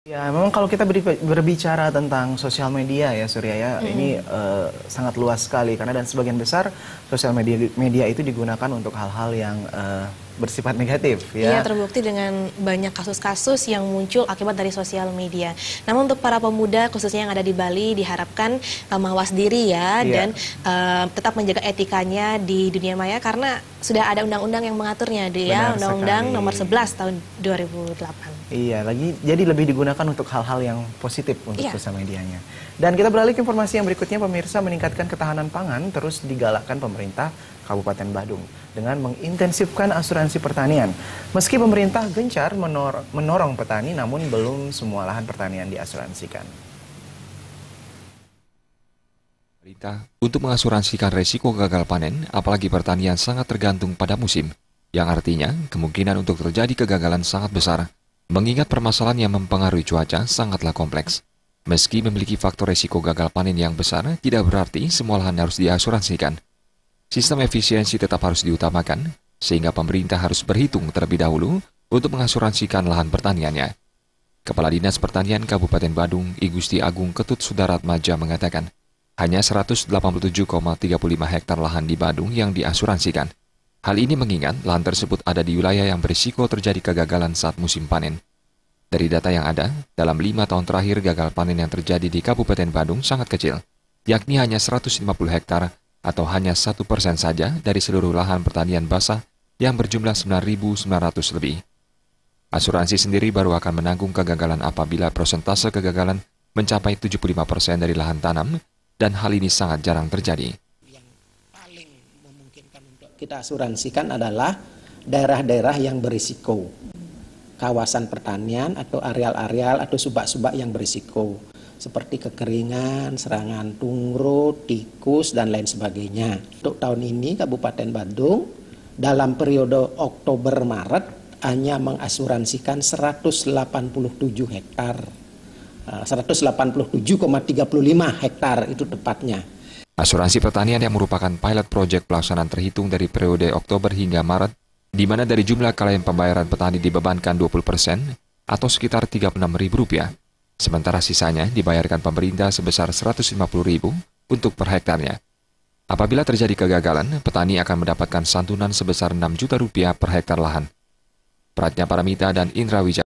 LAHAN PERTANIAN BELUM DIASURANSIKAN
MENINGKATKAN KETAHANAN PANGAN TERUS DIGALAKKAN PEMERINTAH KABUPATEN BADUNG, DENGAN MENGINTENSIFKAN ASURANSI PERTANIAN. MESKI PEMERINTAH GENCAR MENORONG PETANI, NAMUN BELUM SEMUA LAHAN PERTANIAN DIASURANSIKAN.